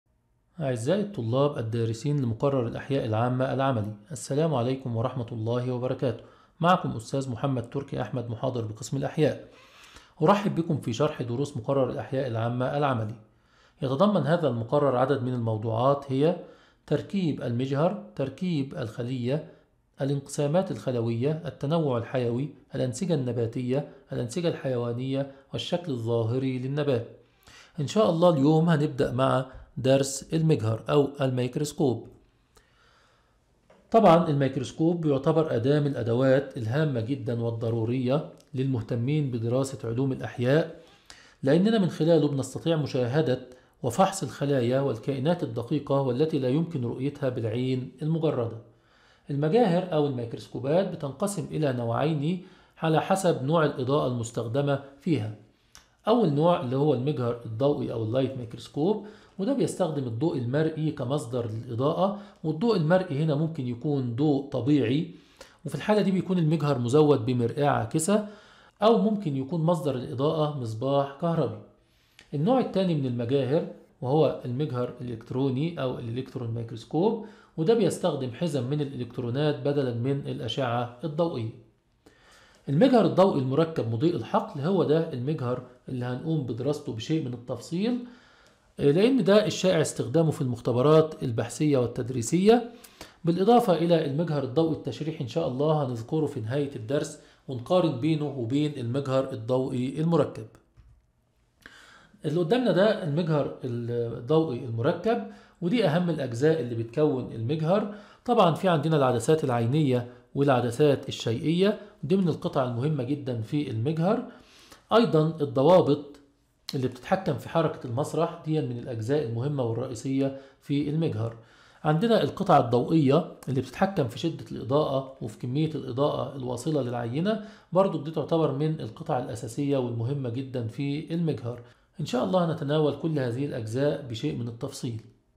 الأحياء العامة | عملي | 1-1 | مقدمة عن المجهر الضوئي المركب
المقررات المفتوحة - الأحياء العامة العملي (101حيا-4 )
الوحدة 1 : المجهر The Microscope
الدرس 1 :  مقدمة عن المجهر الضوئي المركب
1- مقدمة عن محتويات المقرر.
2- أهمية المجهر.
3- أجزاء المجهر الضوئي المركب.